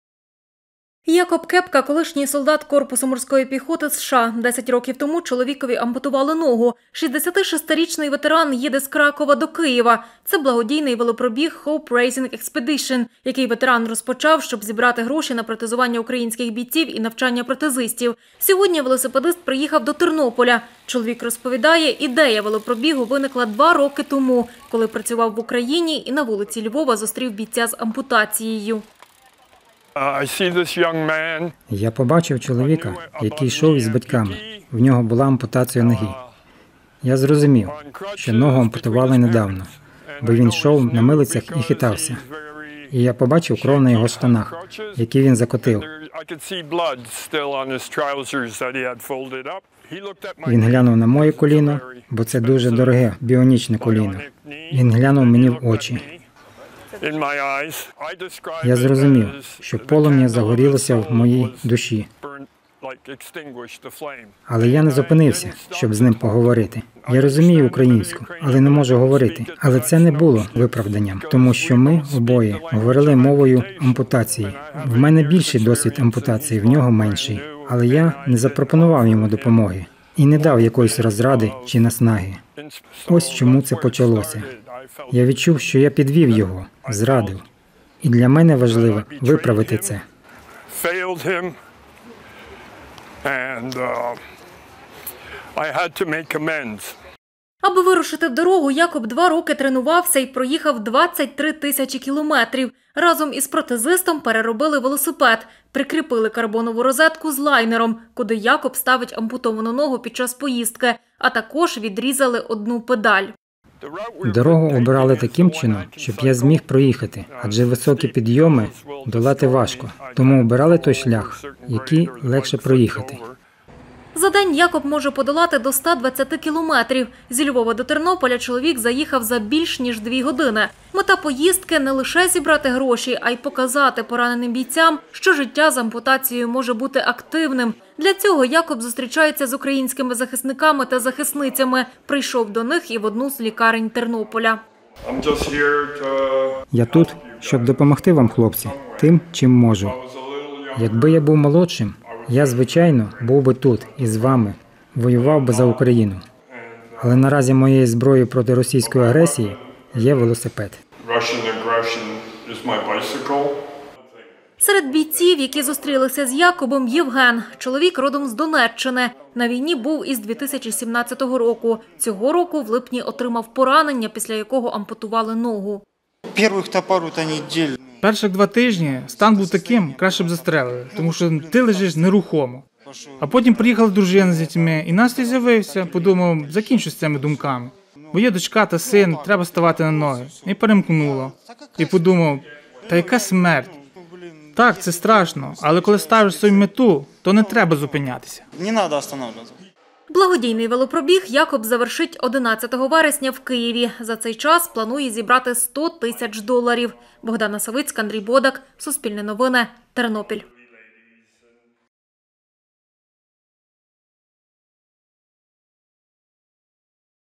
Ветеран без ноги Якоб Кепка долає 1000 кілометрів на велосипеді
Ветеран США з ампутованою ногою їде 1000 кілометрів на велосипеді, щоб зібрати гроші на протезування українських бійців і навчання протезистів. Розпочав свій маршрут Якоб Кепка із Кракова, адже має польське коріння, а завершить поїздку в Києві. За цей час хоче зібрати 100 тисяч доларів. Сьогодні велосипедист приїхав до Тернополя. З ним зустрілася Богдана Савицька.